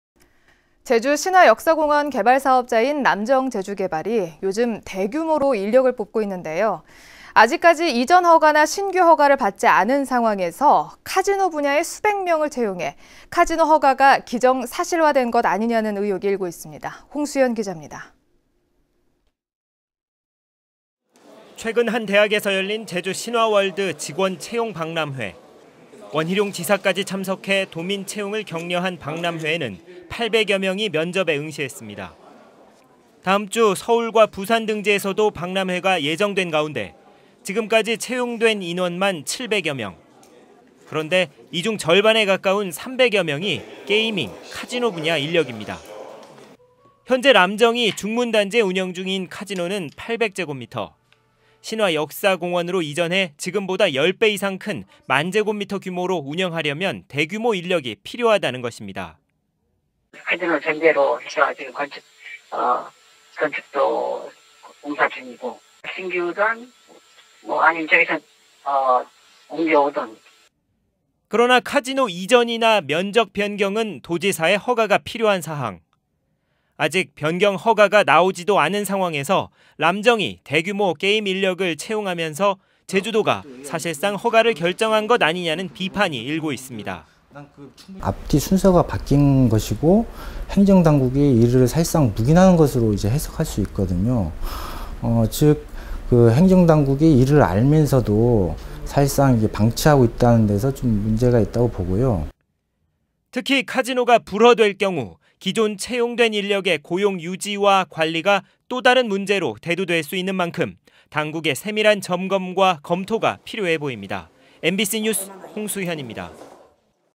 제주MBC 리포트 대규모 카지노 인력 채용 허가는 기정사실 홍수현
◀ＡＮＣ▶
 제주신화역사공원 개발사업자인
람정제주개발이 요즘
대규모로 인력을 뽑고 있는데요.

 아직까지 이전 허가나
신규 허가를 받지 않은 상황에서
카지노 분야에 수 백 명을 채용해
카지노 허가가 기정사실화된 것 아니냐는
의혹이 일고 있습니다.

 홍수현 기자입니다.

           ◀ＥＮＤ▶
           ◀ＶＣＲ▶

 최근 한 대학에서 열린
제주신화월드 직원 채용박람회.

 원희룡 지사까지 참석해
도민 채용을 격려한 박람회에는
800여 명이 면접에 응시했습니다.

 다음주 서울과 부산 등지에서도
박람회가 예정된 가운데,
지금까지 채용된 인원만 700여 명.

 그런데 이 중 절반에 가까운 300여 명이
게이밍, 카지노 분야 인력입니다.

 현재 람정이 중문단지에 운영중인 카지노는
800제곱미터.

 신화역사공원으로 이전해
지금보다 10배 이상 큰 만 제곱미터 규모로
운영하려면
대규모 인력이 필요하다는 것입니다.

           ◀ＳＹＮ▶람정관계자
 "카지노를 전제로 건축도 공사중이고,
신규든 이쪽에서 옮겨오든 (진행할 것이다.)"

 그러나 카지노의 이전이나
면적 변경은
도지사의 허가가 필요한 사항.

 아직 변경 허가가 나오지도 않은 상황에서
람정이 대규모 게임 인력을 채용하면서
제주도가 사실상 허가를 결정한 것 아니냐는
비판이 일고 있습니다.

           ◀ＩＮＴ▶제주주민자치연대
 "앞뒤 순서가 바뀐 것이고 행정당국이 이를
사실상 묵인하는 것으로 해석할 수 있어 당국이
알면서도 방치하는 문제가 될 수 있다 보고요."

 특히 카지노가 불허될 경우
기존 채용된 인력의 고용 유지와 관리가
또 다른 문제로 대두될 수 있는 만큼
당국의 세밀한 점검과 검토가 필요해 보입니다.

 mbc news 홍수현입니다.